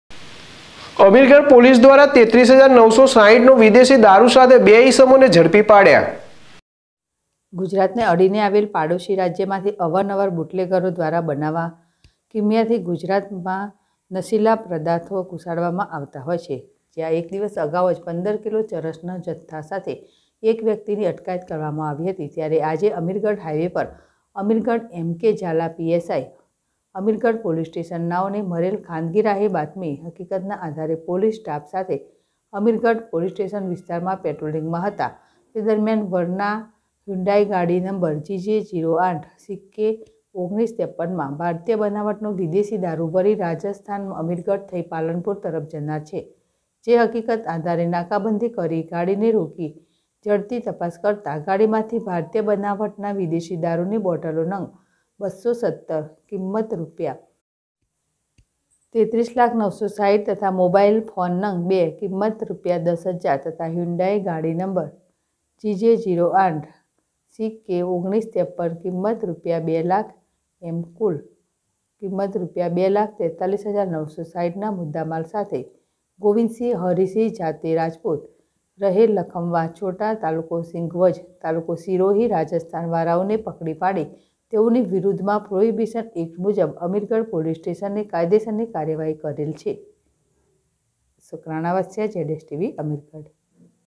ગુજરાતને અડીને આવેલ પડોસી રાજ્ય માંથી અવાર નવાર બુટલેગરો દ્રારા અવનવા કિમીયાથી
અમીરગઢ હ્યુન્ડાઇ વર્ના ગાડી નંબર- GJ08CK1953 માંથી ભારતીય બનાવટનો વિદેશી દારૂની બોટલ નંગ-૨૧૭ કી.રૂ.૩૩,૯૬૦ ની હેરાફેરી કરતાં ગાડી સાથે કુલ કી.રૂ.૨,૪૩,૯૬૦/- ના મુદ્દામાલ સાથે બે ઇસમને પકડી પાડતી અમીરગઢ પોલીસ